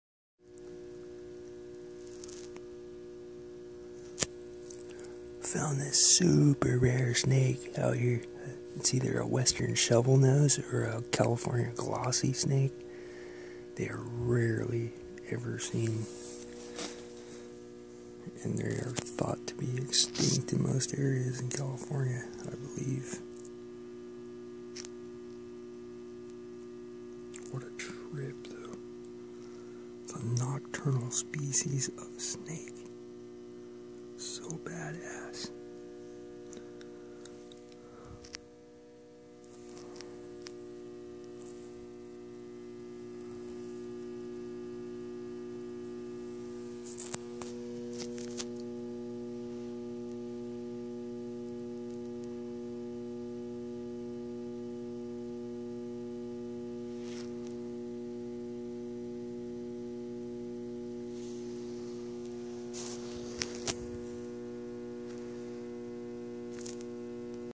Snake saved for release
California Nightsnake is a rear fanged mildly toxic nocturnal species that rarely bites.